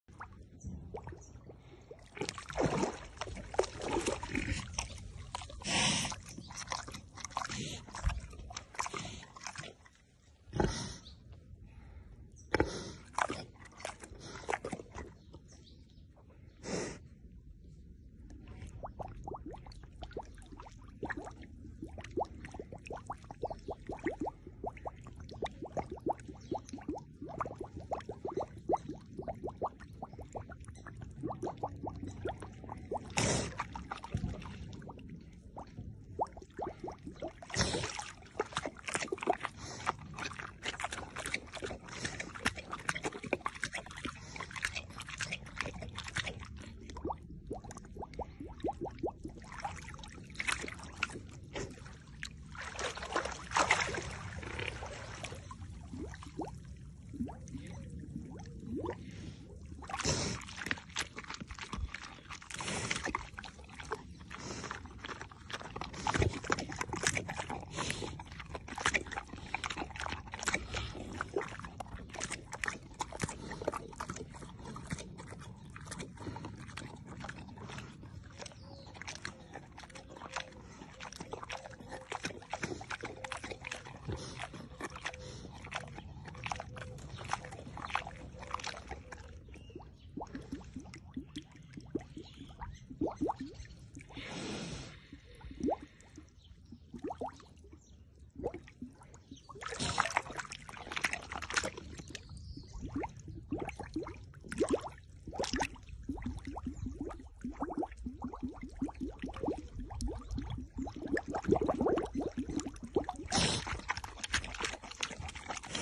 Agustín refrescándose y haciendo pompitas en el agua.
Vuelven unos días de calor y Agustín aprovecha para pasar un buen rato refrescándose mientras hace pompitas en el agua.
Aquí te dejamos el vídeo para que disfrutes casi tanto como disfruta él.
Fundación Santuario Vegan ❤️🐷 (y a todos los animales).

Ayúdanos a seguir salvando vidas y dándoles los cuidados que necesitan y un nuevo hogar aquí en Fundación Santuario Vegan 💚